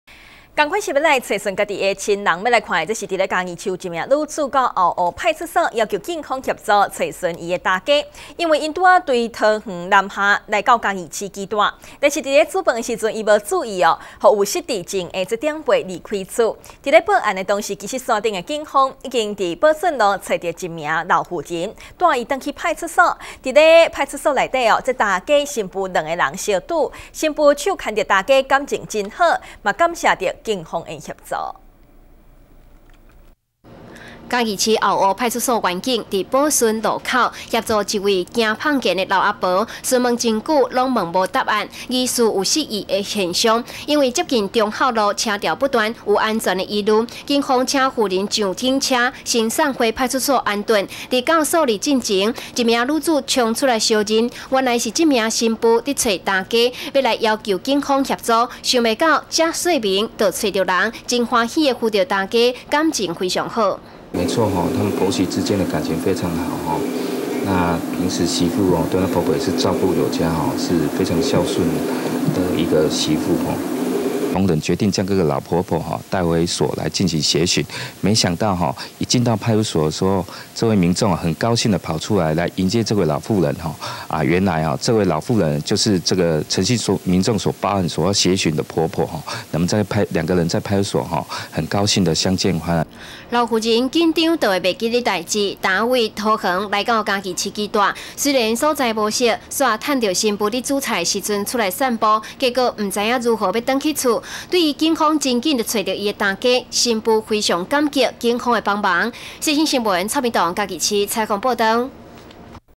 世新新聞 媳婦找婆婆 報案巧遇警尋人回所
媳婦找婆婆 報案巧遇警尋人回所

嘉義市一名女子到後湖派出所要求警方協尋他的婆婆，因為她們剛從桃園南下居住，在煮飯時不注意讓有失憶的長輩離家，在報案當時，線上警方已自保順路尋到一位走失婦人，才帶回派出所，在所前，婆媳兩人相遇，媳婦手扶著婆婆感情非常好，感謝警方協助。